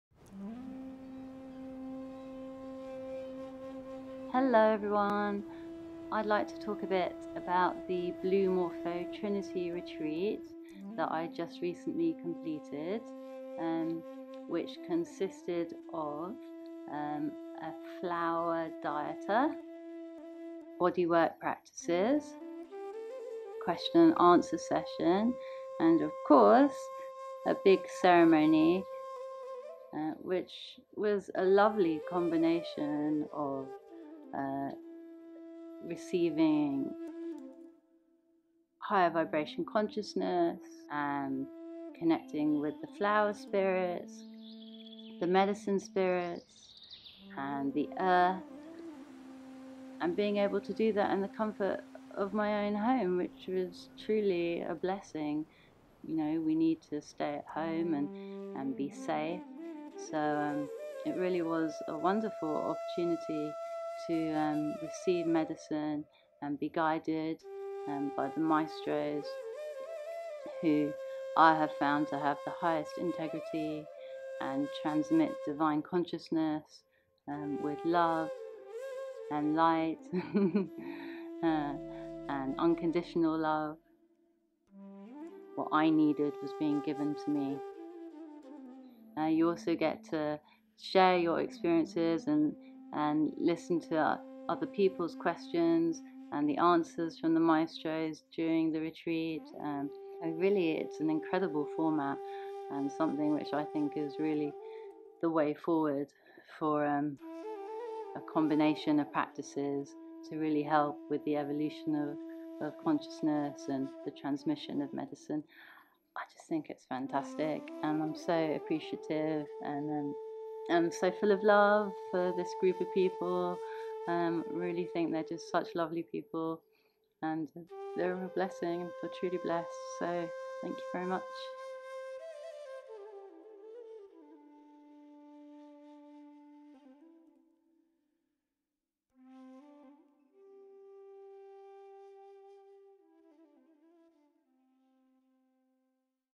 Blue Morpho Triniti Retreats - Isabel shares her experience
Isabel shares some of her Triniti Retreat Experience.
Treat yourself to a consciousness reset!